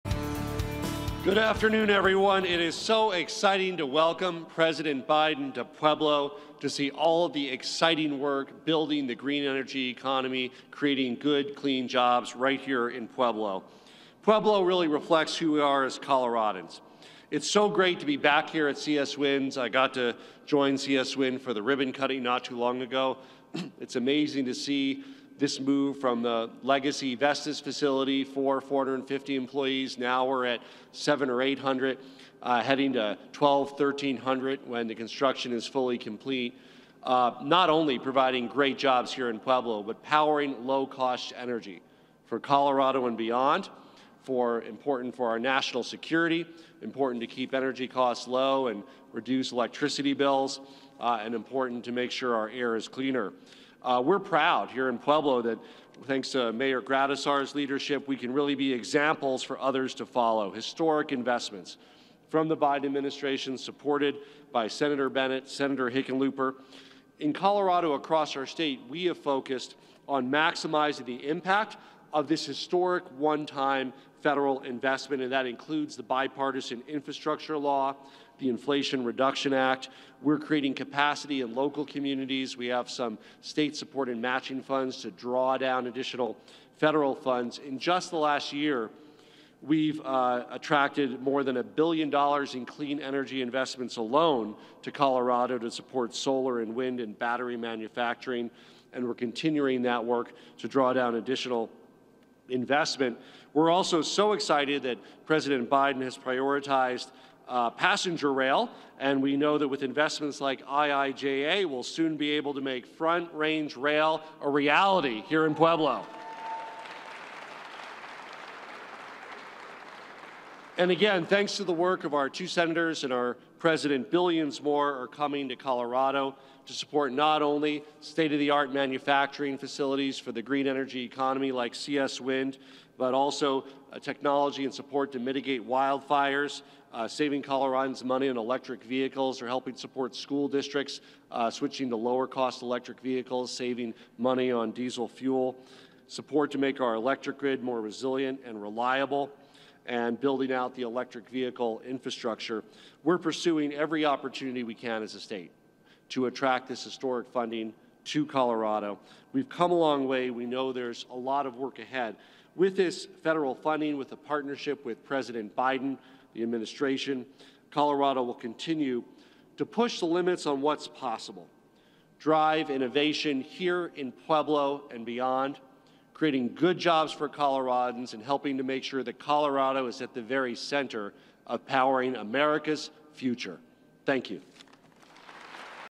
WATCH: Governor Polis talks Biden's visit to Pueblo at CS Wind
CO Governor Jared Polis welcomed President Joe Biden to Pueblo Tuesday afternoon.